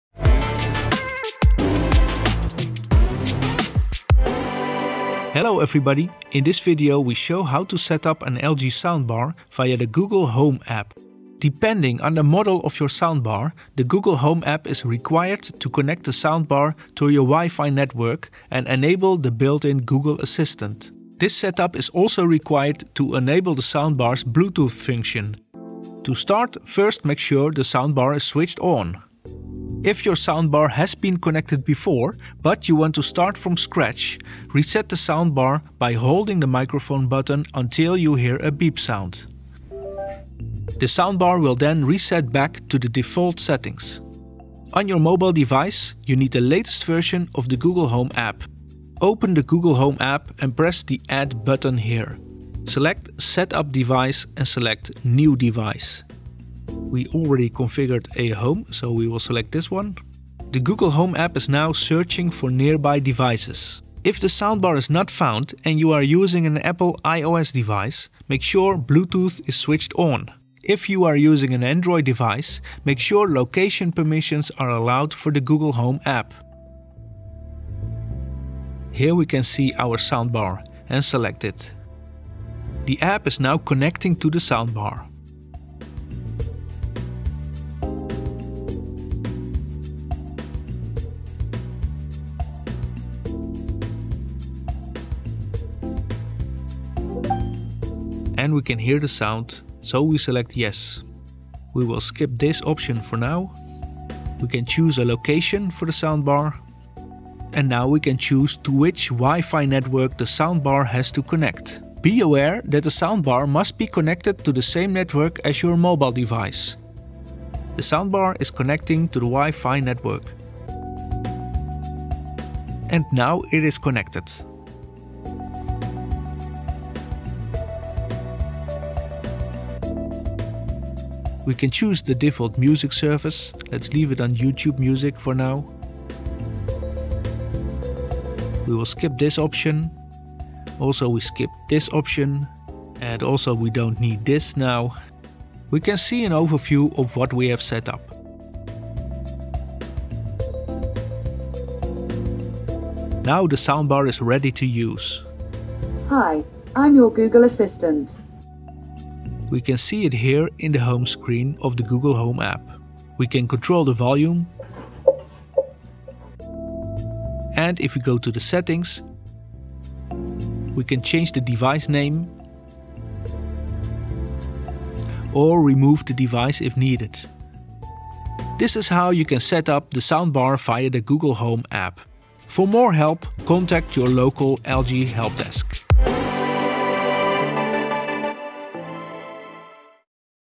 Soundbar LG | Come configurare la Soundbar con l'applicazione Google Home
Video guida per come utilizzare e configurare una soundbar LG con l'app Google Home.
 Su alcuni modelli di soundbar LG, ciò è necessario per connettere la soundbar alla rete WiFi e abilitare la funzione Bluetooth.

00:00 Introduzione
00:26 Cosa fare prima
00:49 Aggiunta della soundbar all'app Google Home
01:04 Cosa controllare se la soundbar non viene trovata
01:47 Connessione alla rete WiFi
02:32 La soundbar è pronta per l'uso